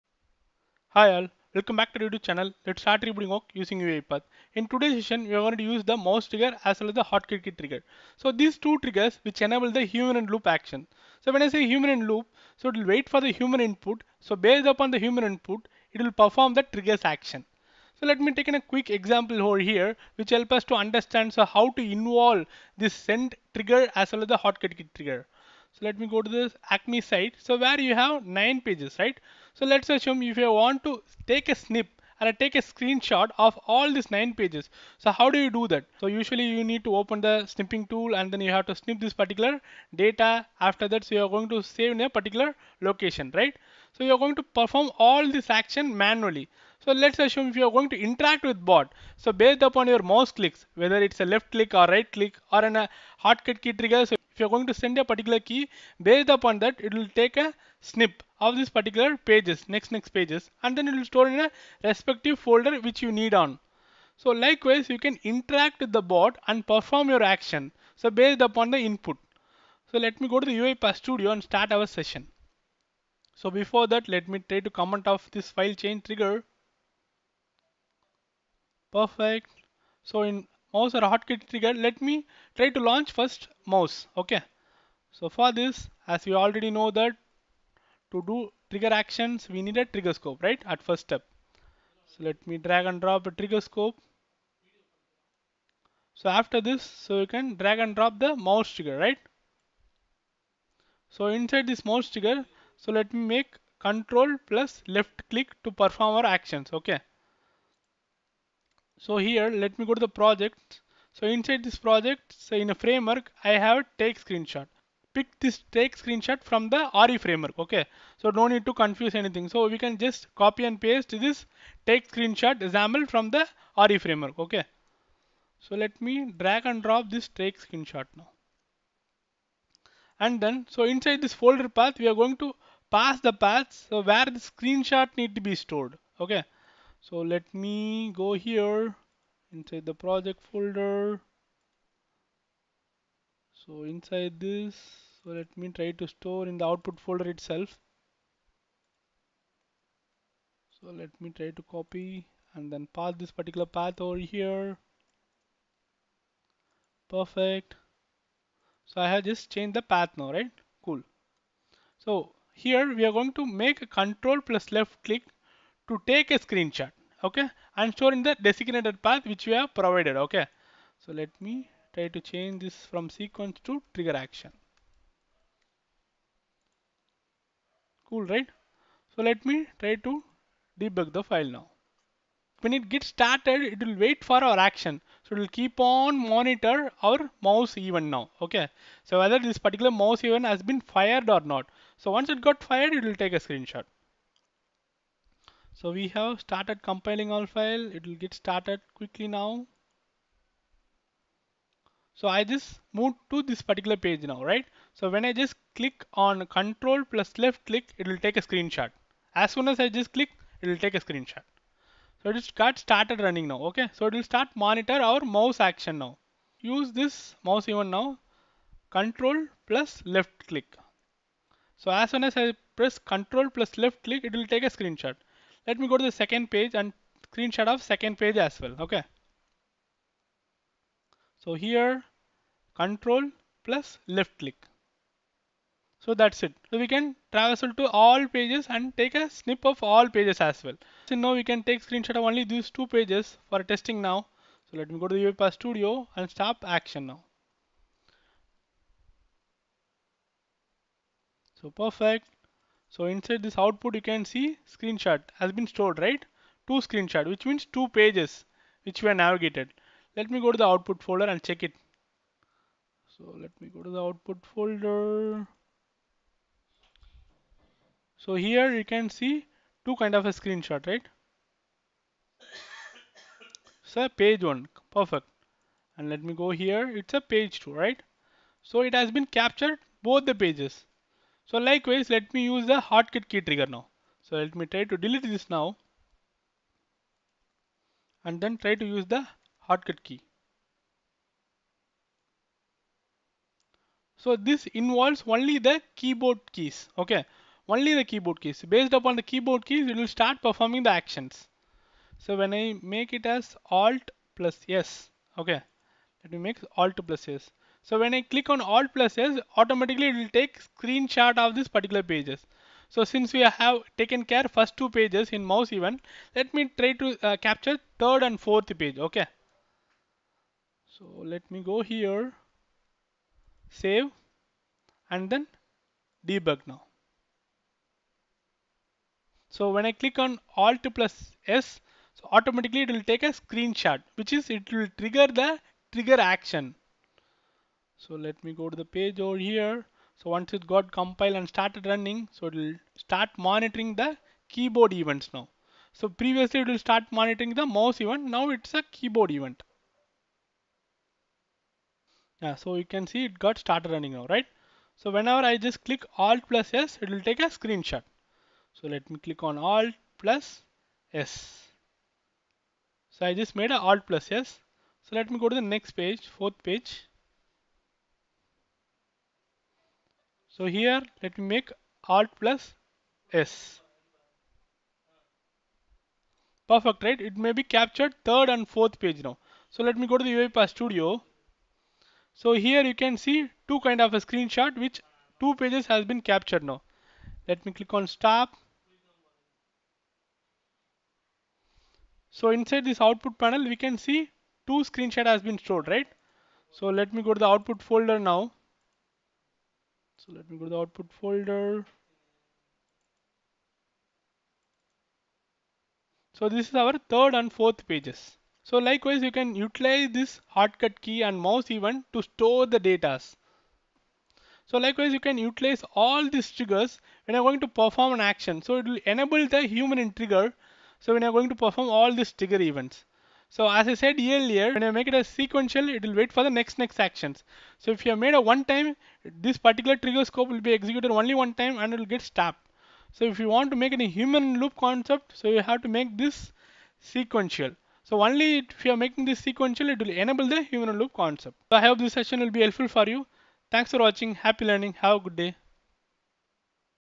UiPath Tutorial l Trigger - Send & Hotkey Trigger
Trigger Scope - Basically this trigger scope will help to start/resume the action once it met the criteria.

Send & Hotkey Trigger can be used either inside a Trigger Scope activity or by itself within a trigger workflow. When used by itself.

Example: When we need to take a screenshot of site which have 10 Page. We can use Send & Hotkey Trigger to perform the action using human Mouse click/ Keyword Strokes which involves human in loop.

Happy Learning !!